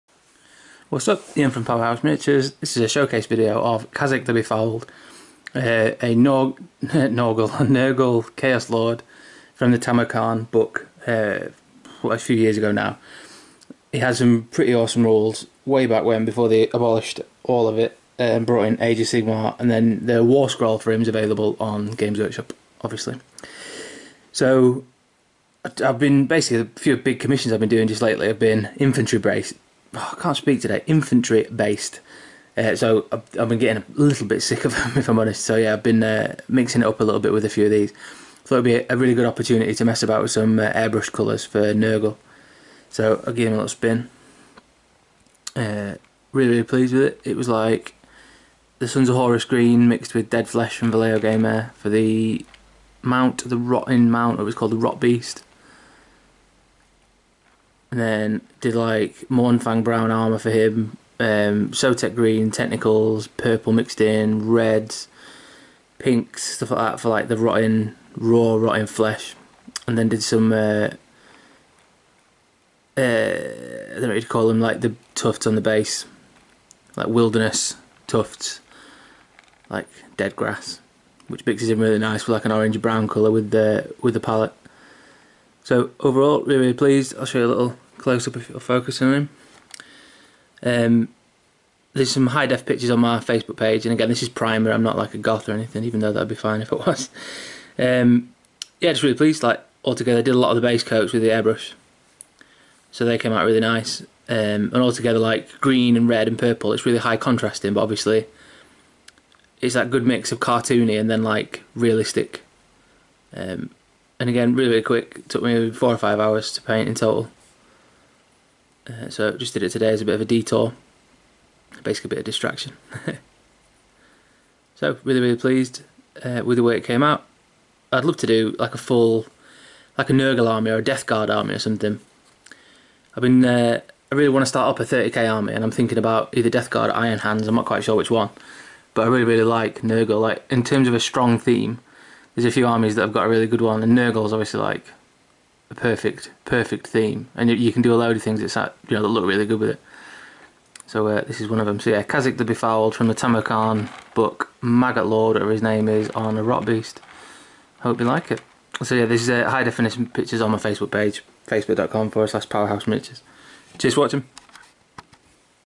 Warhammer Fantasy Kazyk The Befouled Showcase - Pro Painted
This guy would make an awesome conversion for a Chaos Space Marine Chaos Lord on bike or demonic steed!